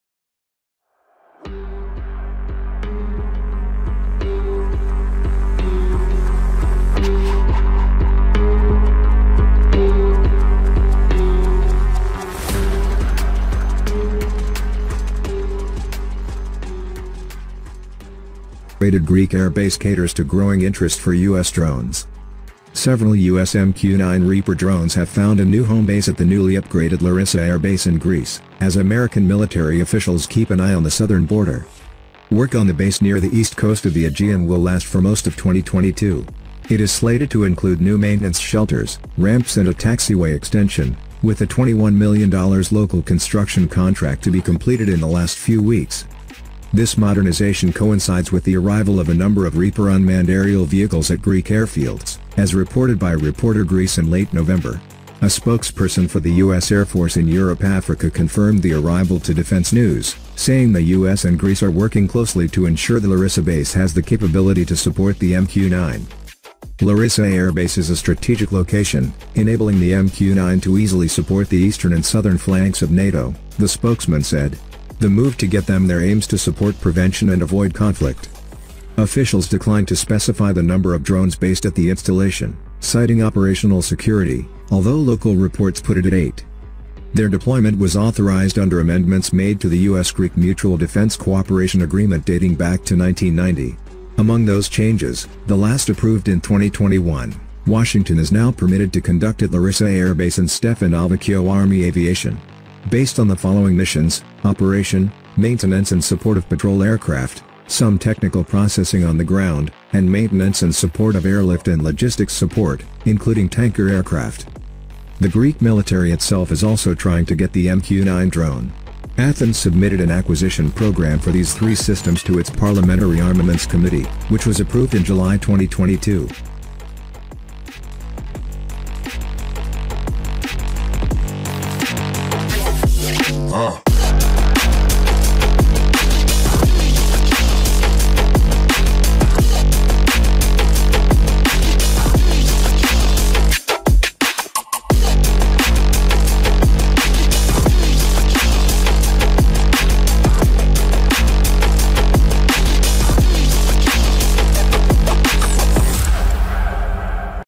Upgraded Greek air base caters to growing interest for US drones
This modernization coincides with the arrival of a number of Reaper unmanned aerial vehicles at Greek airfields, as reported by Reporter Greece in late November. A spokesperson for the US Air Force in Europe-Africa confirmed the arrival to Defense News, saying "the US and Greece are working closely to ensure the Larissa base has the capability" to support the MQ-9.
Larissa Air Base is a strategic location, enabling the MQ-9 to easily support the eastern and southern flanks of NATO, the spokesman said. The move to get them there aims to support prevention and avoid conflict.
Their deployment was authorized under amendments made to the US-Greek Mutual Defense Cooperation Agreement dating back to 1990. Among those changes, the last approved in 2021, Washington is now permitted to conduct at Larissa Air Base and Stefanovikio Army Aviation. Based on the following missions: operation, maintenance and support of patrol aircraft, some technical processing on the ground, and maintenance and support of airlift and logistics support, including tanker aircraft.

The Greek military itself is also trying to get the MQ-9 drone. Athens submitted its acquisition program of these three systems to its Parliamentary Armaments Committee.